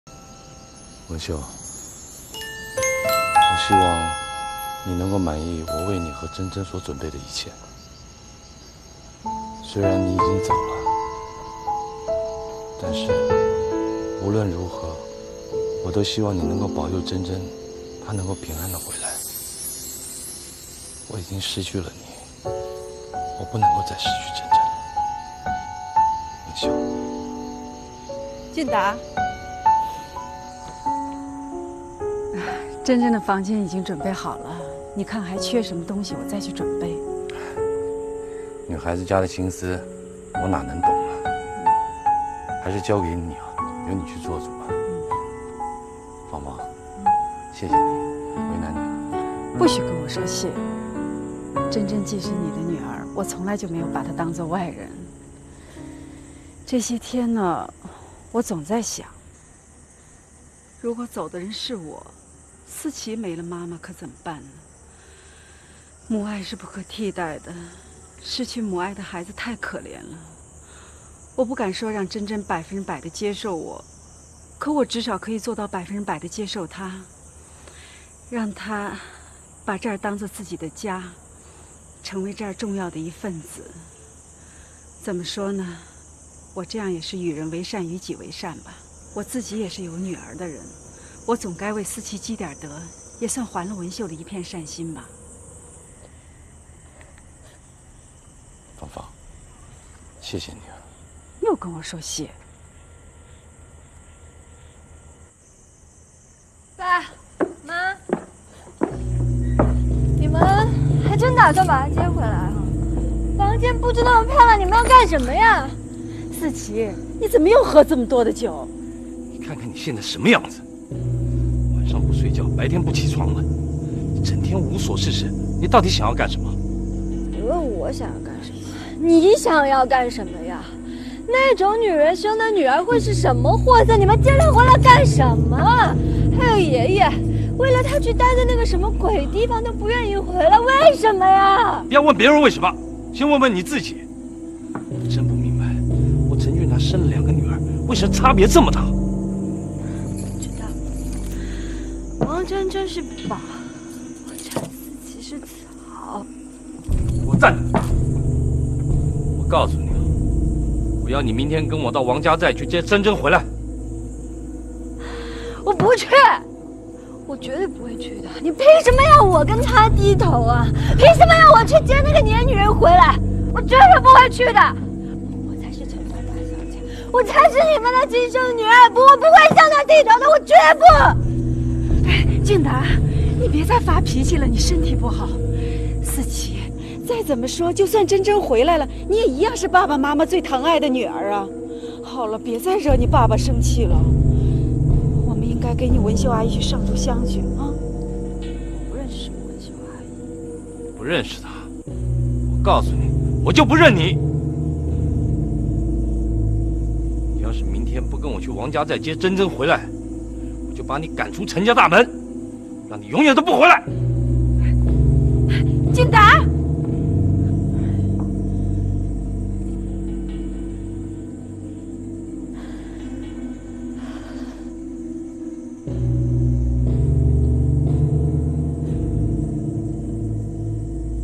丈夫明目張膽把私生女接回家，妻子忍氣吞聲，一旁的女兒怒了：我才是你親女兒！EP22
簡介：未婚母親文繡，為了報復戀人的背叛，在嫉妒和憤恨中將兩家女嬰偷偷對換，讓自己的女兒變成豪門千金，從而引發了跨越兩代長達數十年的種種恩怨情仇的故事。
該片段來自電視劇。